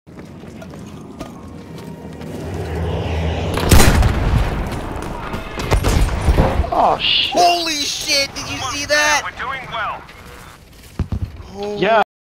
Plane Takedown
First time getting that one-hit panzerfaust kill on a plane.